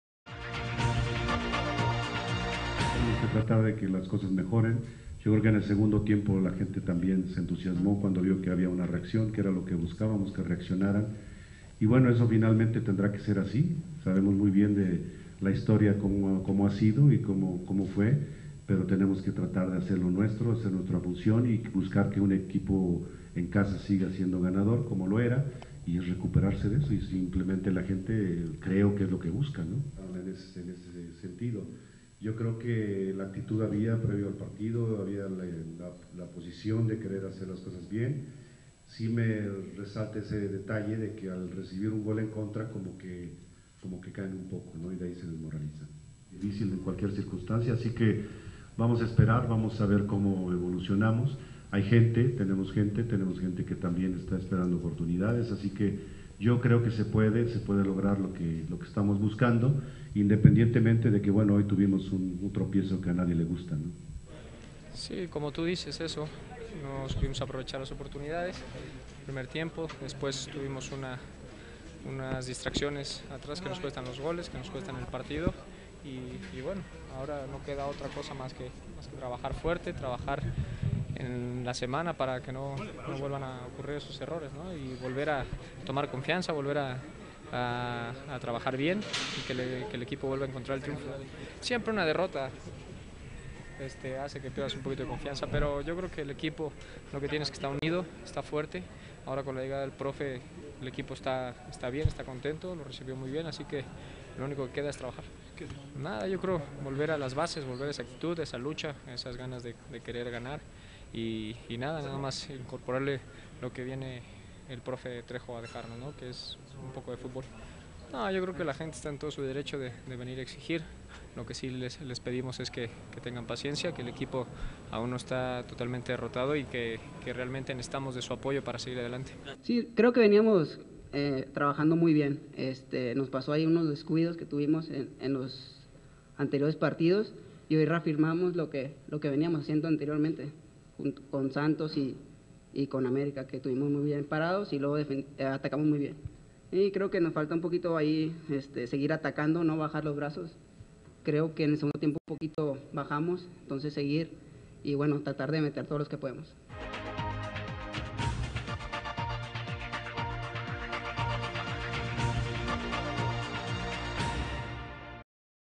Puebla vs Tigres Apertura 2010 reacciones
Los camoteros del Puebla pierderon 1-3 ante los Tigres de la Universidad Autónoma de Nuevo León en duelo correspondiente a la jornada 8 del Torneo Apertura 2010 de la Primera División del futbol mexicano, el debut de José Luis Trejo como director técnico de La Franja no fue como esperaba ya que su ecuadra sucumbió ante la escuadra de Ricardo Ferreti.